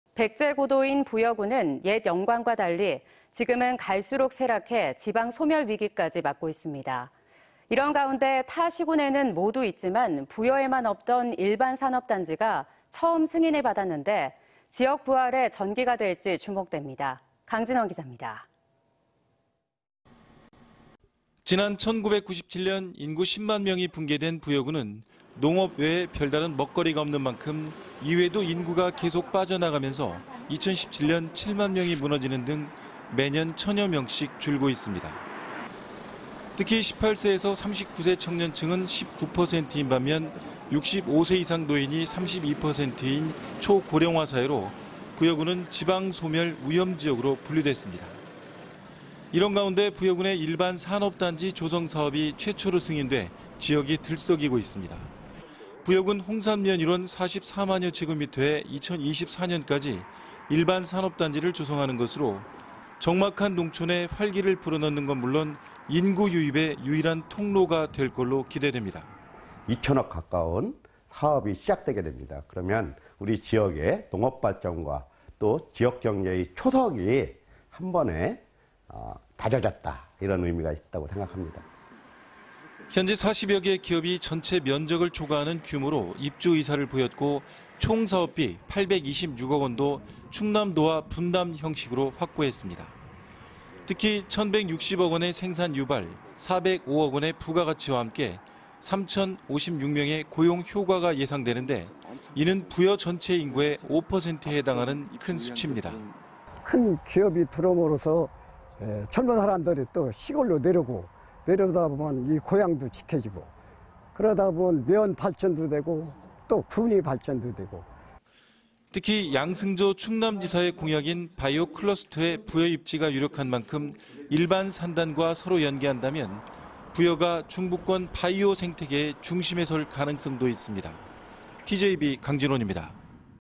[TJB뉴스]부여 첫 일반산단 승인 젊은 도시 될까
【 앵커멘트 】

백제 고도인 부여군은
옛 영광과 달리
지금은 갈수록 쇠락해
지방소멸 위기까지
맞고 있습니다.

이런 가운데,
타 시군에는 모두 있지만
부여에만 없던
일반 산업 단지가
처음 승인을 받았는데
지역 부활의
전기가 될 지 주목됩니다.

강진원 기자.